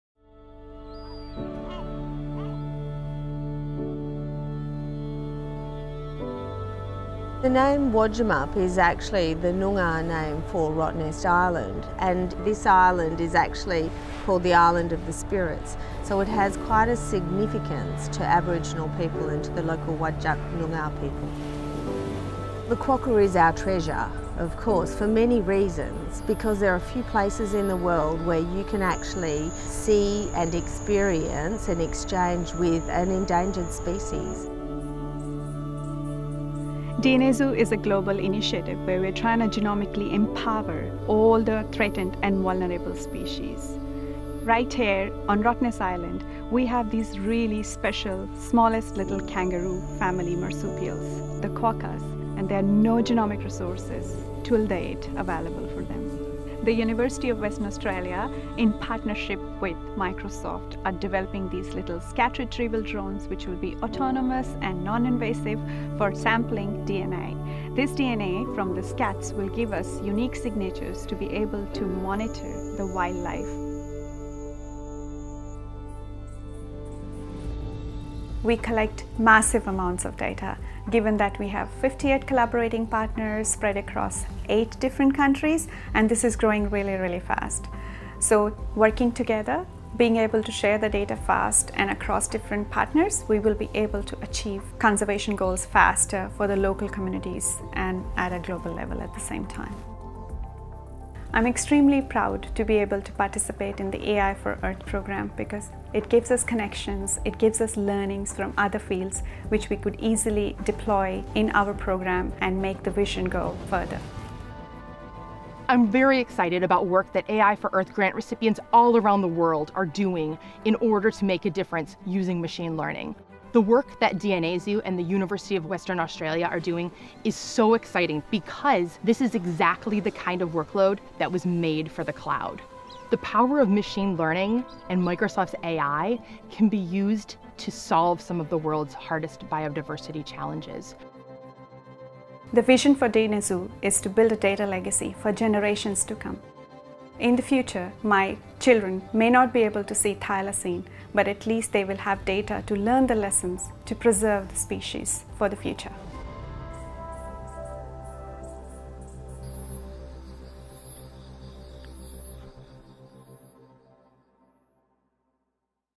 AI to Save the Quokka
With the help of Microsoft's AI for Earth grant, the University of Western Australia is working to study and protect the quokka through the use of drones, AI and the cloud.